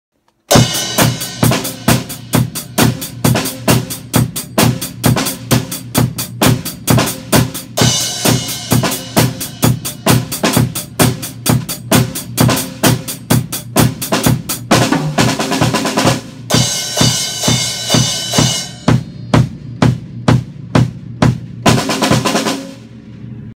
群馬県、伊勢崎市、ドラム教室、レッスン風景【カリフラワーさん】
カリフラワーさんのレッスン風景です。
無料体験レッスン実施中！

平田ドラム教室
群馬県伊勢崎市本町4-37 K-SOUND2F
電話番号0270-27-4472